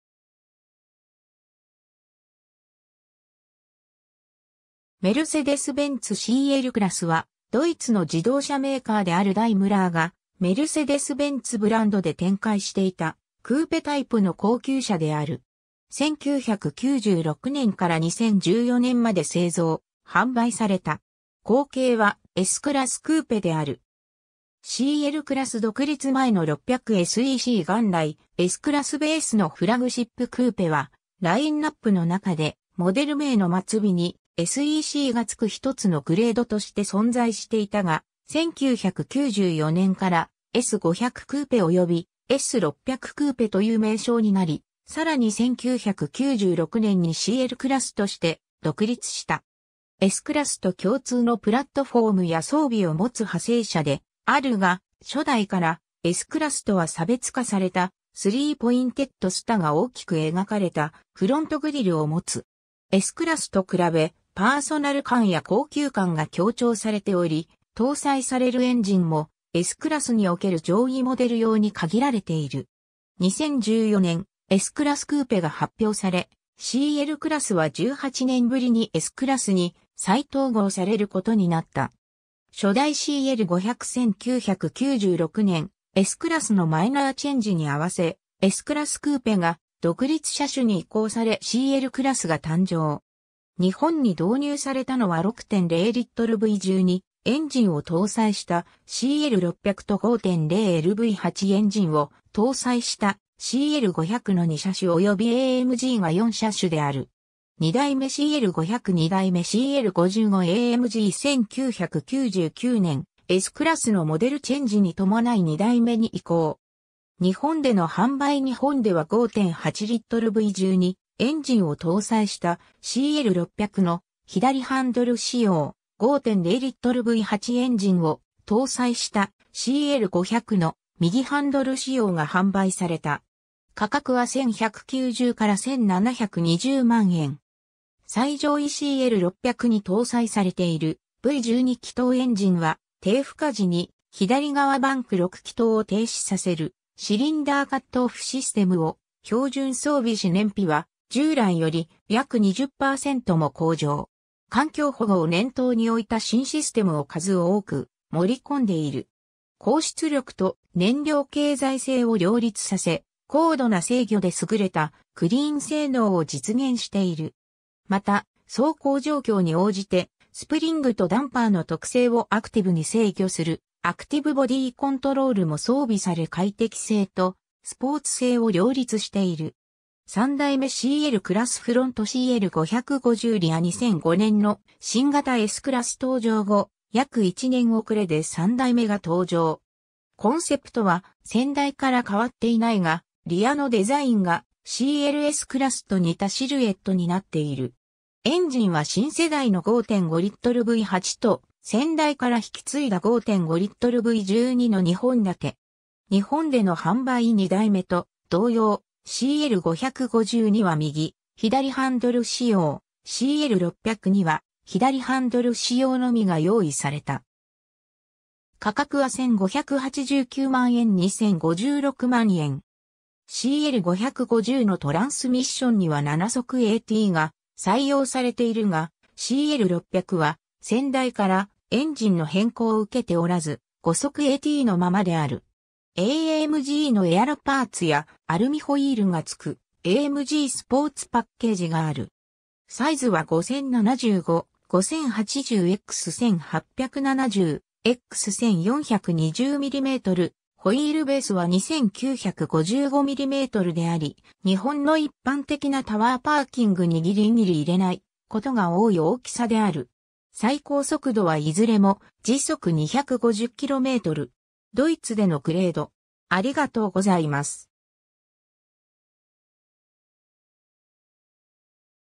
メルセデス・ベンツ・CLクラス
メルセデス・ベンツ・CLクラス（Mercedes-Benz CL-Class ）は、ドイツの自動車メーカーであるダイムラーがメルセデス・ベンツブランドで展開していたクーペタイプの高級車である。
1996年から2014年まで製造・販売された。
後継はSクラスクーペである。
 CLクラス独立前の600SEC 元来、Sクラスベースのフラグシップクーペは、ラインナップの中でモデル名の末尾に「SEC」が付く1つのグレードとして存在していたが、1994年から「S500クーペ」および「S600クーペ」という名称になり、さらに1996年にCLクラスとして独立した。
Sクラスと共通のプラットフォームや装備をもつ派生車であるが、初代からSクラスとは差別化された、スリーポインテッド・スターが大きく描かれたフロントグリルを持つ。
Sクラスと比べ、パーソナル感や高級感が強調されており、搭載されるエンジンもSクラスにおける上位モデル用に限られている。
 2014年、Sクラスクーペが発表され、CLクラスは18年ぶりにSクラスに再統合されることになった。
 初代CL500 1996年、Sクラスのマイナーチェンジに併せ、Sクラスクーペが独立車種に移行されCLクラスが誕生。
日本に導入されたのは6.0L V12エンジン（M120）を搭載した「CL600」と5.0LV8エンジン（M119 5.0）を搭載した「CL500」の2車種（ヨーロッパと北米には「CL420」の設定があった）およびAMGが4車種である。
 2代目CL500 2代目CL55 AMG 1999年、Sクラスのモデルチェンジに伴い2代目に移行。
 日本での販売 日本では5.8L V12エンジン（M137）を搭載した「CL600」の左ハンドル仕様、5.0L V8エンジン（M113 E50）を搭載した「CL500」の右ハンドル仕様（のちに左ハンドル仕様も追加された）が販売された。
 価格は1190～1720万円。
最上位CL600に搭載されているV12気筒エンジンは低負荷時に左側バンク 6 気筒を停止させる「シリンダー・カットオフ・システム（ZAS）」を標準装備し燃費は従来より約20%も向上。
環境保護を念頭に置いた新システムを数多く盛り込んでいる。
高出力と燃料経済性」を両立させ、高度な制御で優れたクリーン性能を実現している。
また、走行状況に応じてスプリングとダンパーの特性をアクティブに制御するアクティブボディーコントロールも装備され快適性とスポーツ性を両立している。
 3代目CLクラス フロント CL550 リア 2005年の新型Sクラス登場後、約1年遅れで3代目が登場。
コンセプトは先代から変わっていないが、リアのデザインがCLSクラスと似たシルエットになっている。
エンジンは新世代の5.5L V8（M273）と先代から引き継いだ5.5L V12（M275）の2本立て。
 日本での販売 2代目と同様、「CL550」には右・左ハンドル仕様、「CL600」には左ハンドル仕様のみが用意された。
価格は1589万円～2056万円。
「CL550」のトランスミッションには7速AT（7G-TRONIC）が採用されているが、「CL600」は先代からエンジンの変更を受けておらず、5速ATのままである。
AMGのエアロパーツやアルミホイールがつく「AMGスポーツパッケージ」がある。
サイズは5,075、5,080（AMGスポーツパッケージ）x1,870x1,420mm、ホイールベースは2,955mmであり、日本の一般的なタワーパーキングにぎりぎり入れないことが多い大きさである。
最高速度はいずれも250km/h（スピードリミッター作動）。
 ドイツでのグレード